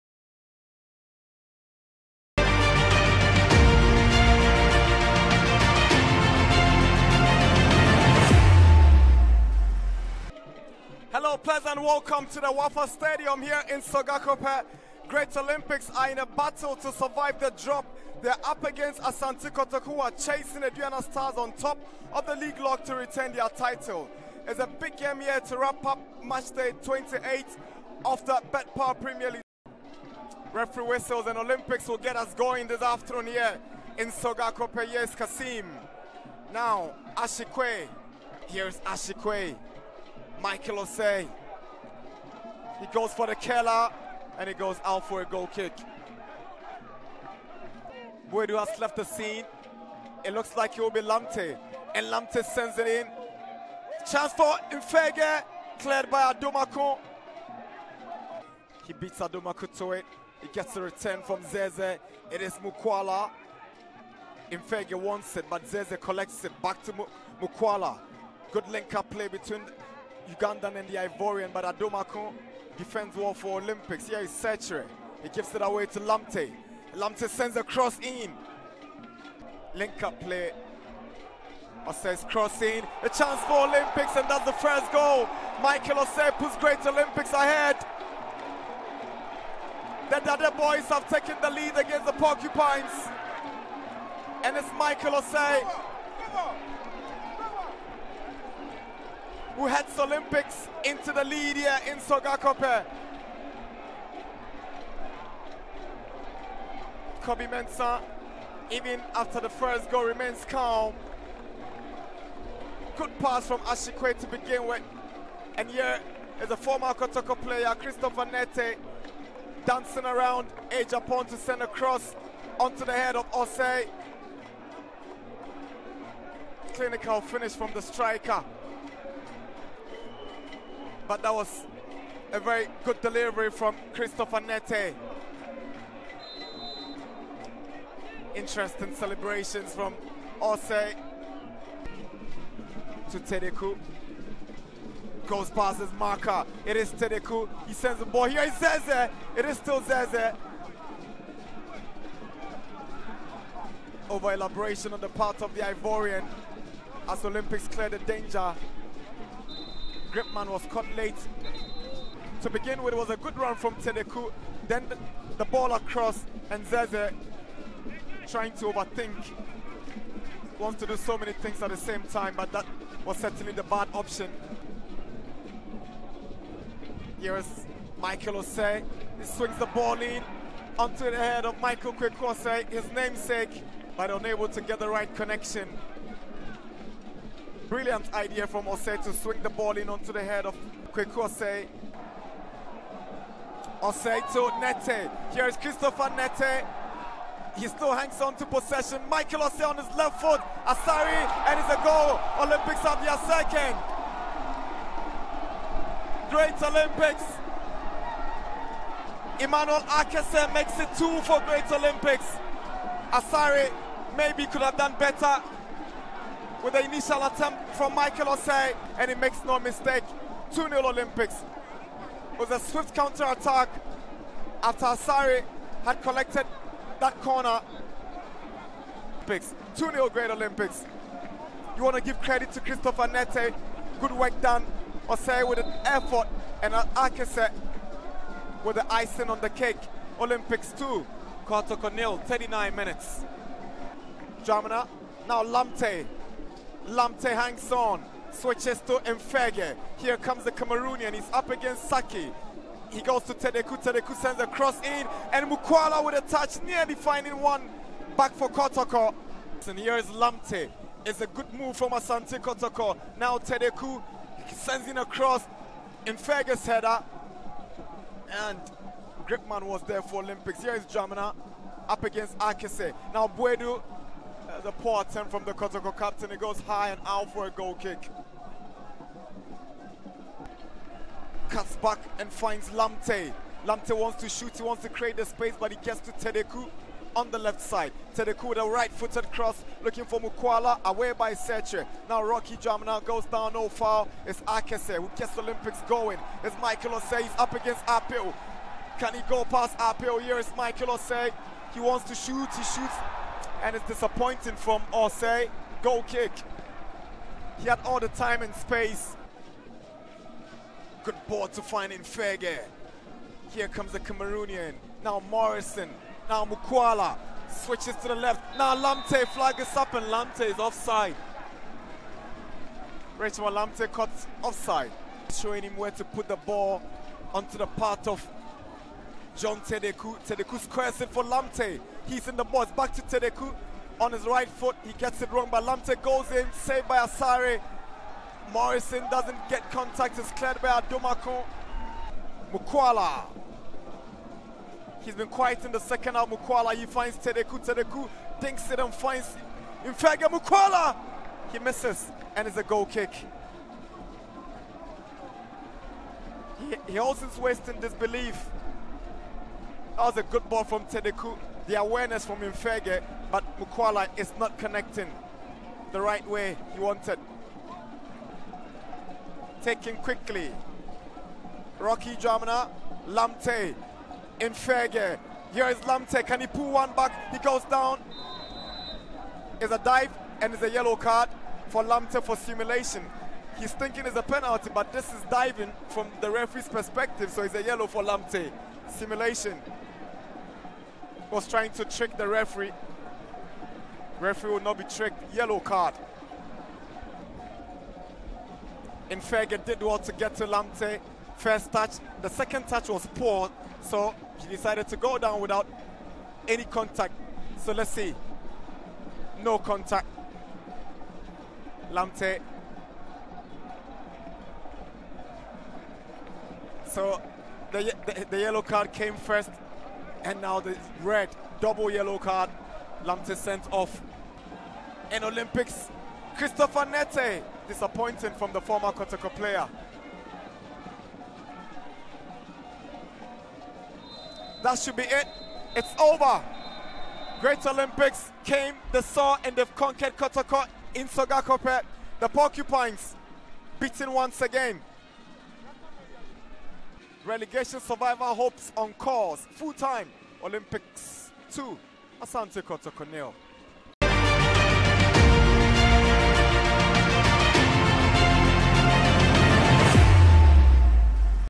Great Olympics 2 : 0 Asante Kotoko | Highlights | betPawa Premier League
In their run to escape relegation, Great Olympics secure a vital win against the defending champions Asante Kotoko. Every loss pushes Kotoko away from the trophy but there is still hope.

Catch more betPawaPremierLeague action on ST Adepa Channel 247 & StarTimes ON App 📲